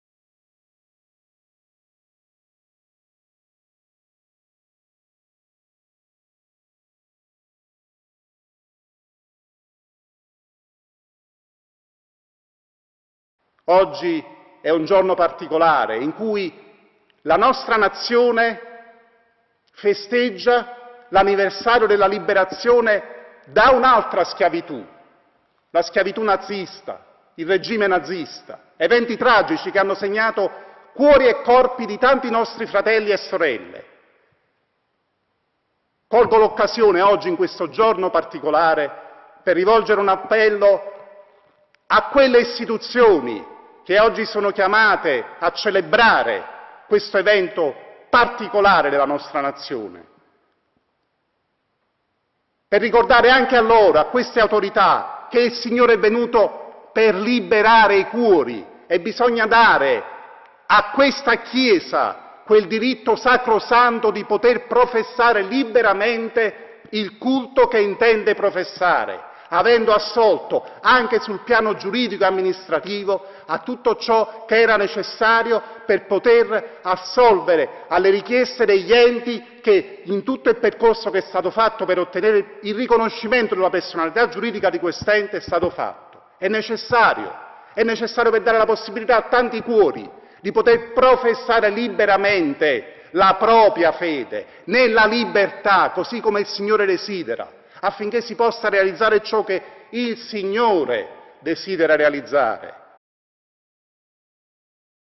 Appello alle autorità civili per il riconoscimento della Chiesa nell'anniversario della Liberazione
Estratto dall'Omelia del 25 aprile 2021
Appello alle autorità civili per il riconoscimento della personalità giuridica della Chiesa nell'anniversario della Liberazione
Vescovo Nello Migliaccio
Responsabile del Dicastero Relazioni Istituzionali